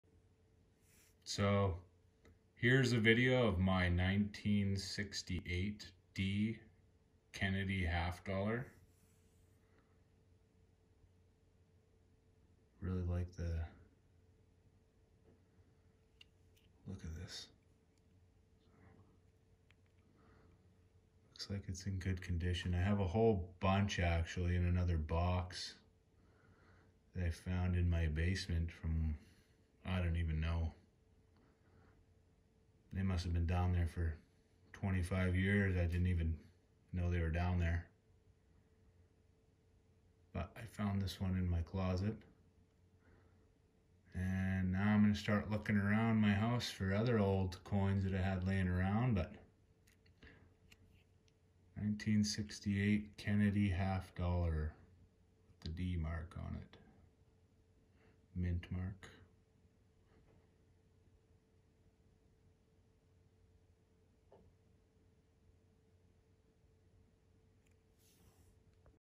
1968 Kennedy d half dollar
This is one of my 1968 Kennedy silver half dollars this is just a short video showing it off just started stacking silver but I also like older coins to so will see Maybey I’ll start collecting and stacking if u want to see more older coins comment in the comments